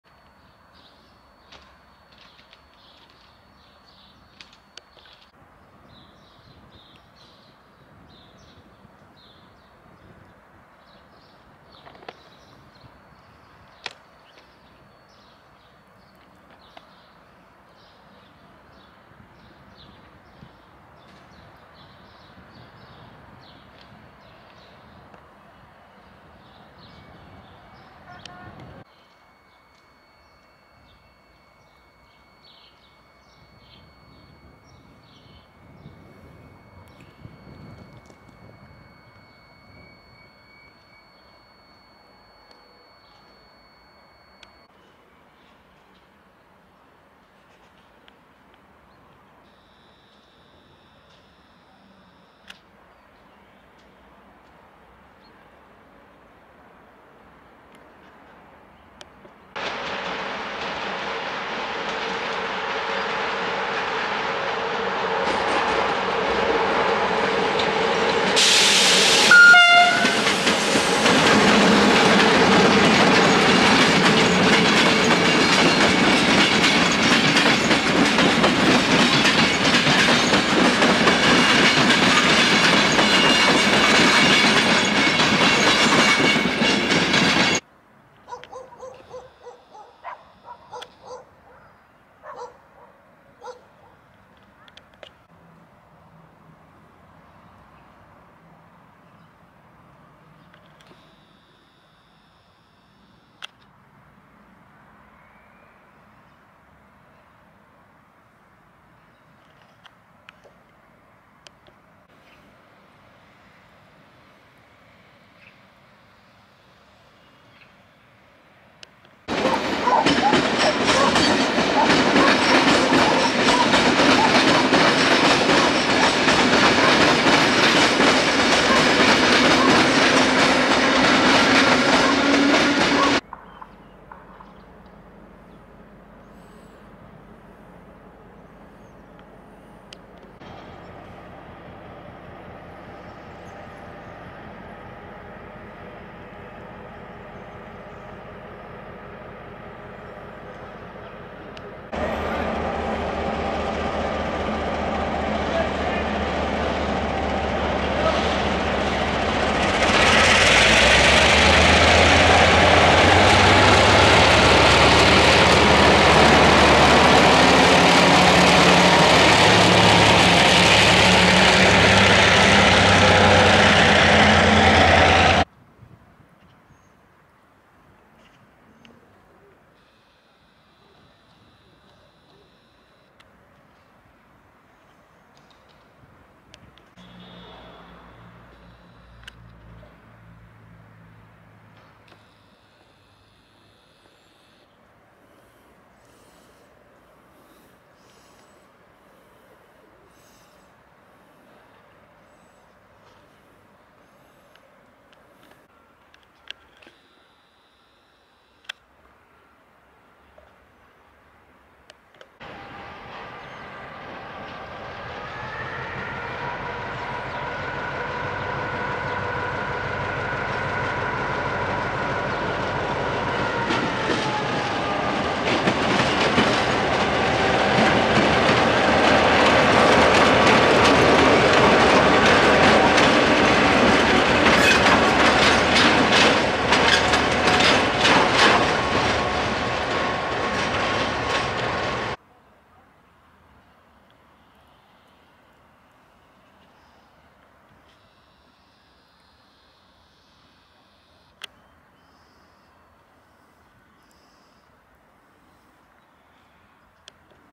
TrainMovie Mini #4 - Podgorica
Short film about the trains of Podgorica.